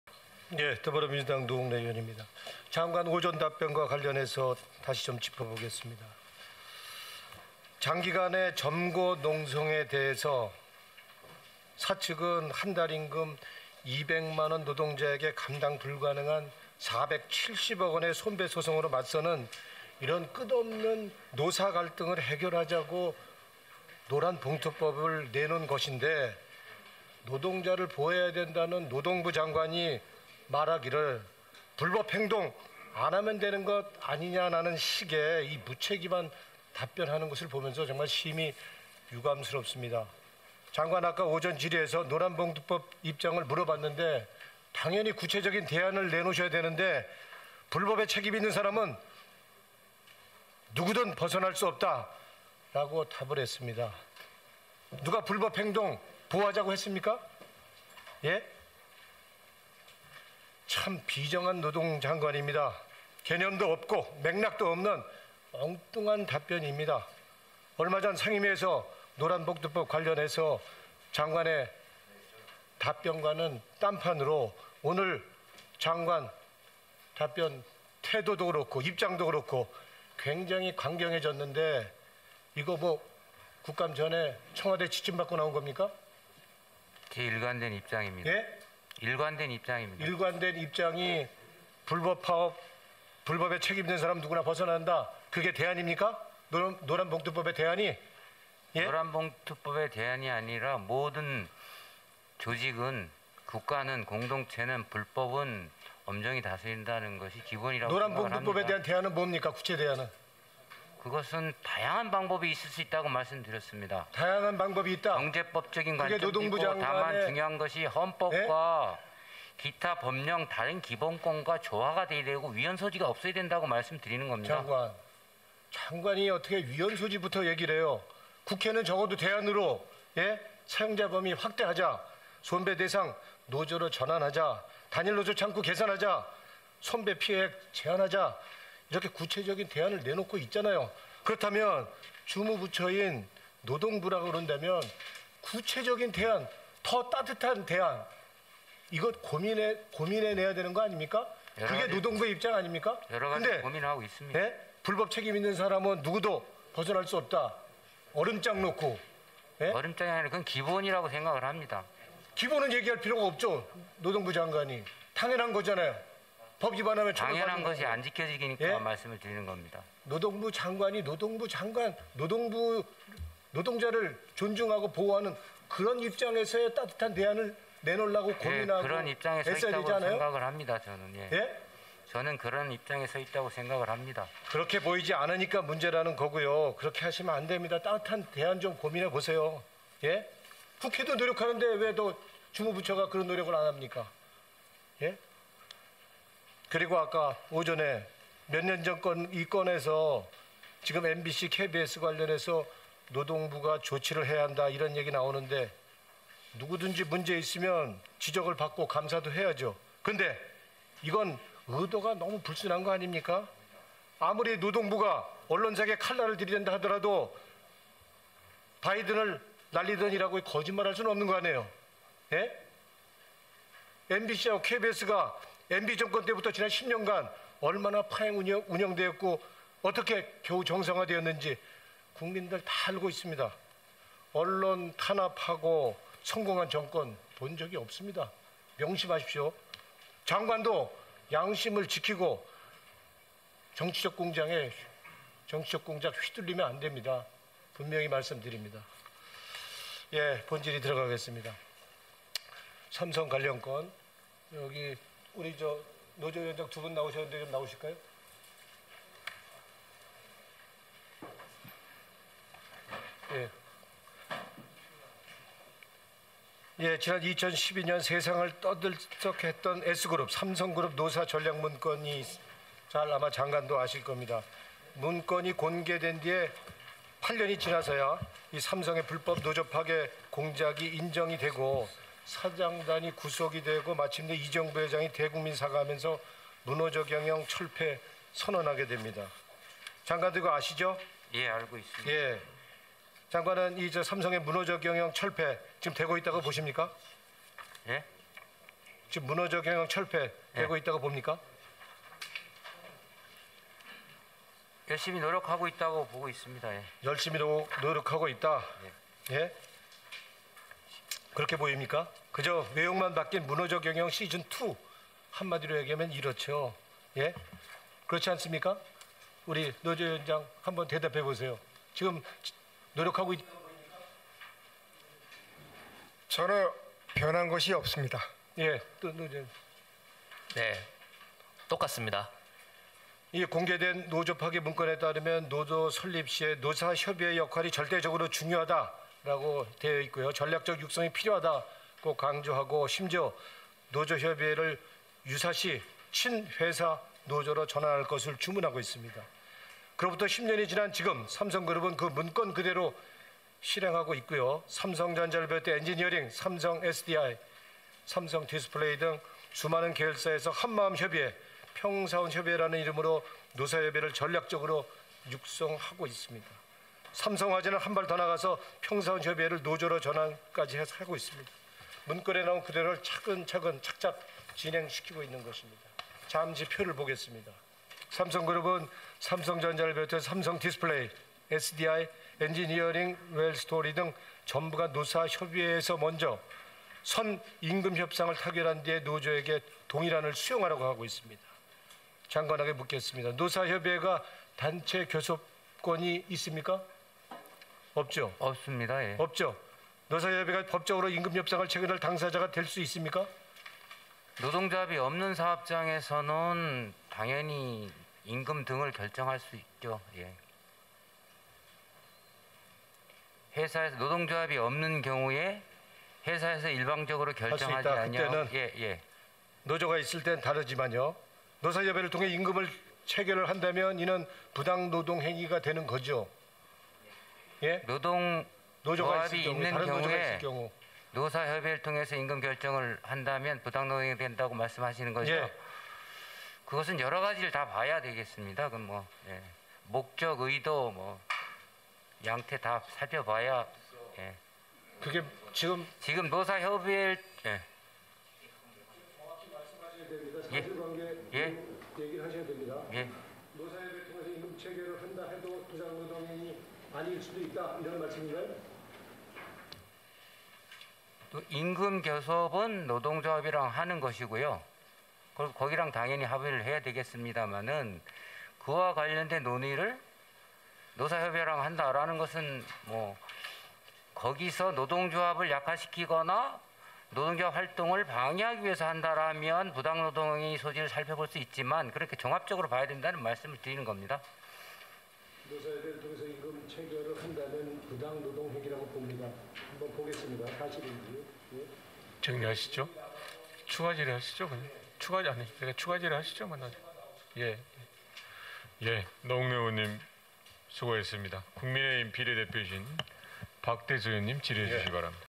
[2022 국정감사] 고용노동부 국정감사 보충질의-노웅래 의원
더불어민주당 민주연구원장인 마포갑 노웅래 국회의원은 2022년 10월 5일 열린 고용노동부 국정감사에서 다음과 같이 질의했습니다.